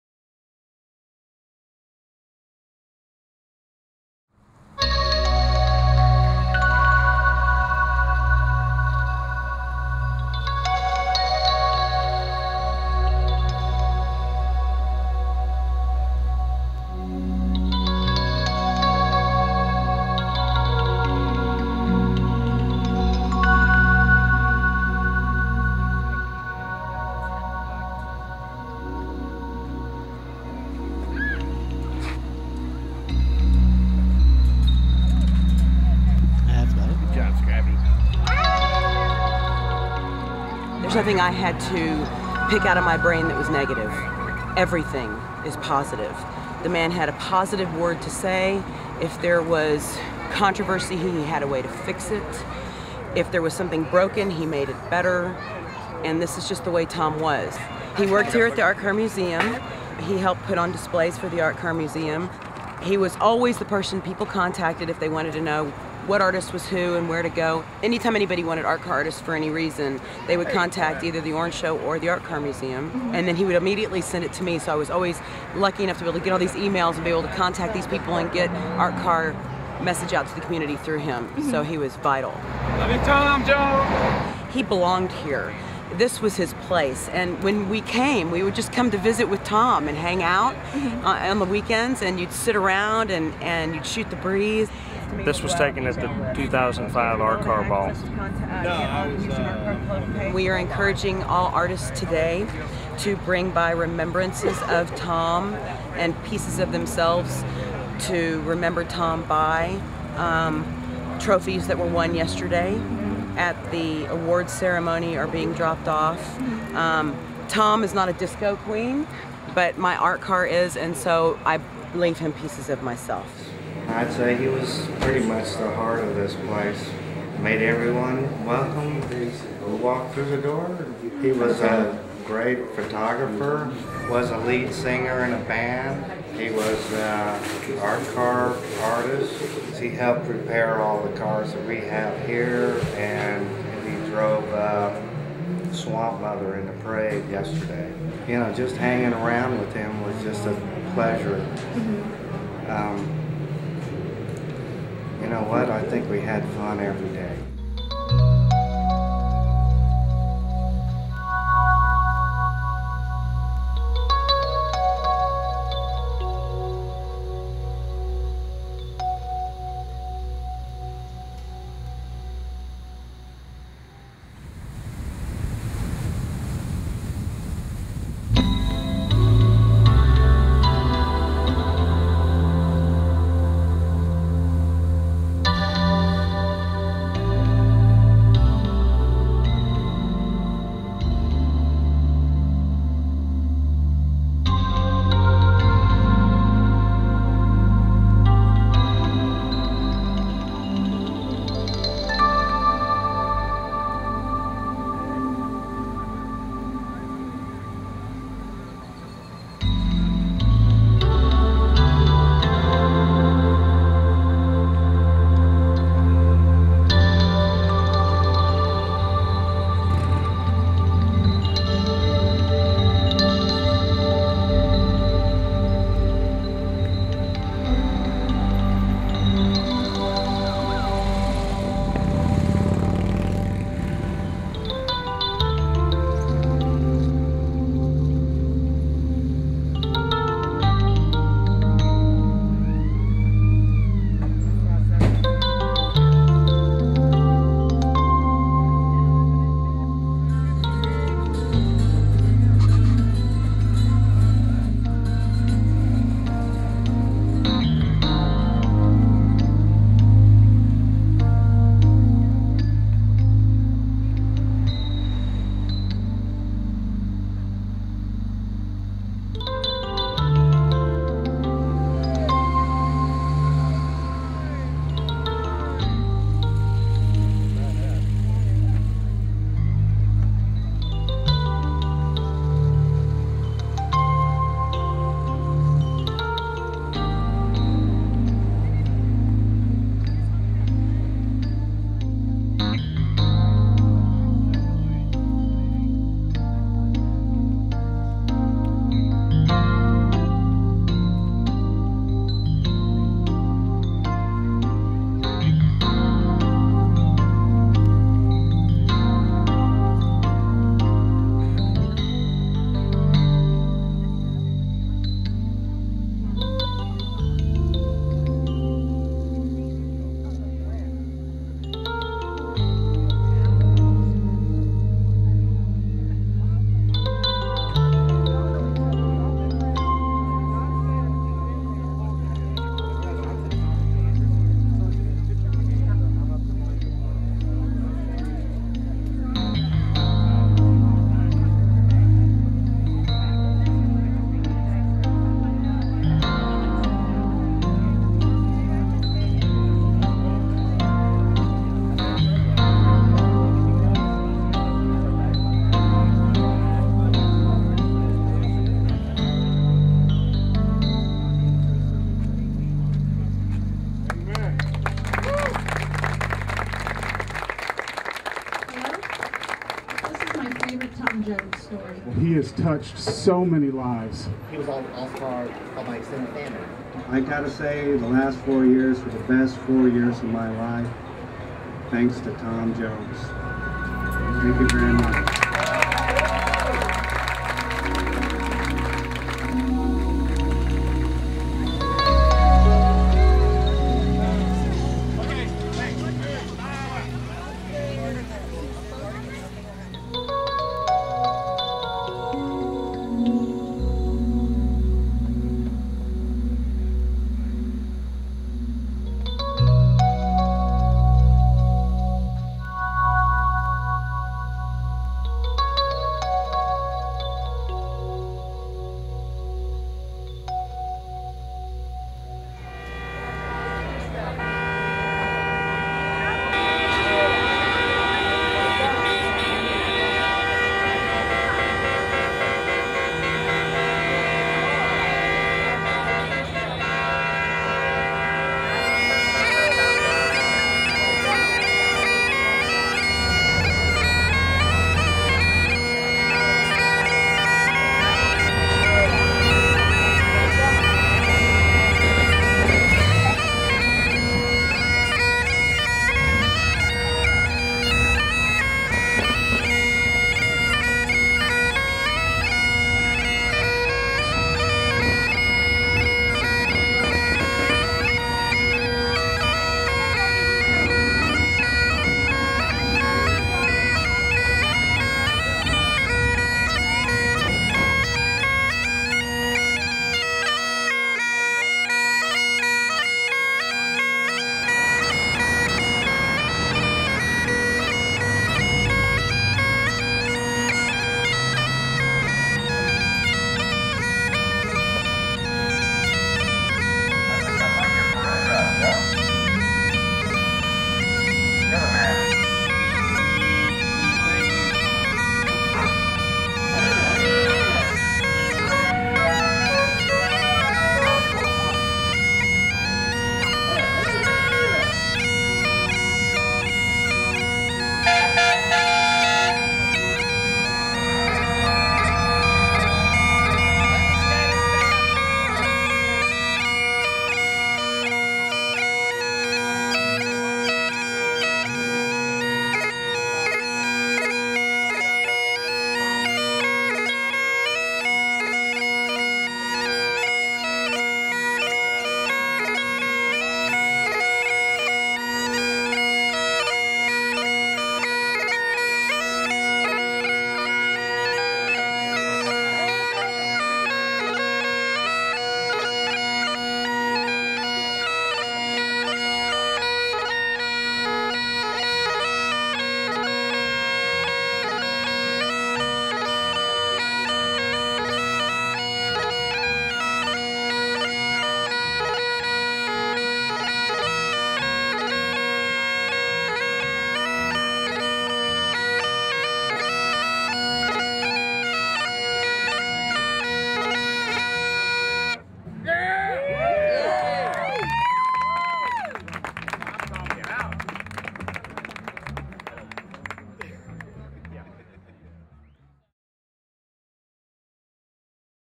an ArtCar Tribute to Tom Jones
Tom Jones was a beloved ArtCar artist, musician and all around good guy. He made a lot of friends working at the Houston Art Car Museum where he was an omnipresent possitive and helpfull influence -- especially to the ArtCar community.